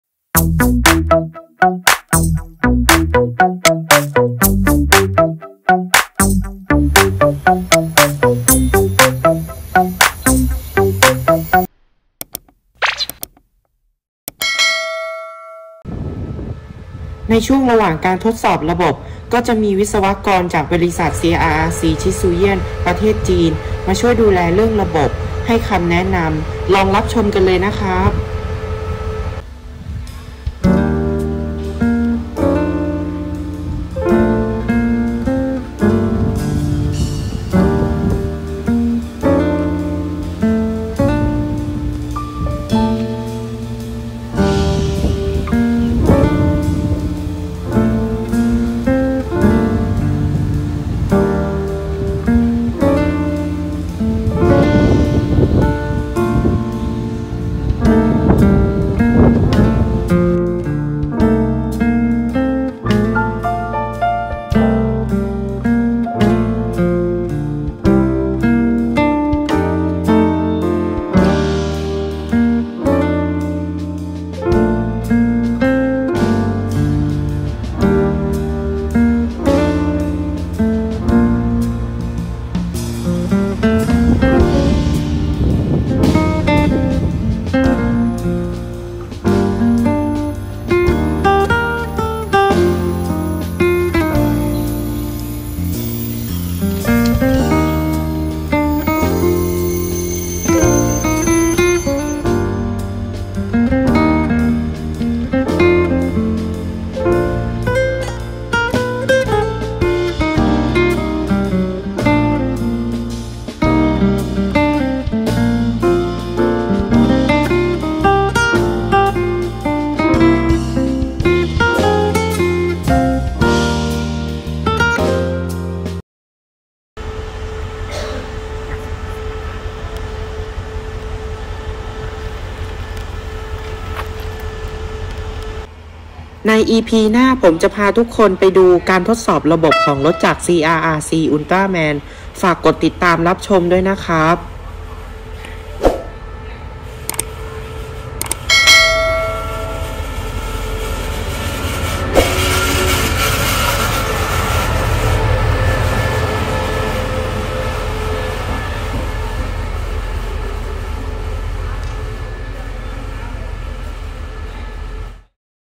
วิศวกรจีนจาก CRRC Qishuyan มาเอง! ช่วยดูแลเรื่องระบบการทดสอบต่างๆ [รถจักรอุลตร้าแมน]
ที่รู้ว่าเป็นวิศวกรจีน เพราะผมได้เข้าไปสอบถามกับเค้าเองครับ เค้าเป็นคนจีน ที่มาจากบริษัทผลิตรถจักรรุ่นนี้โดยเฉพาะมาช่วยดูแลเรื่องระบบการทดสอบต่างๆครับ

ยังไงต้องขออภัยด้วยนะครับ ที่ถ่ายห่างและถ่ายน้อยไปหน่อย ทำให้ภาพไม่ชัด เนื่องจากจะได้ไม่รบกวนการปฏิบัติงานของเจ้าหน้าที่ครับ

ฝากกดติดตามและกดกระดิ่งเพื่อเป็นกำลังใจและรับชมคลิปใหม่ๆจากช่องสกายพาเดินทางด้วยนะครับ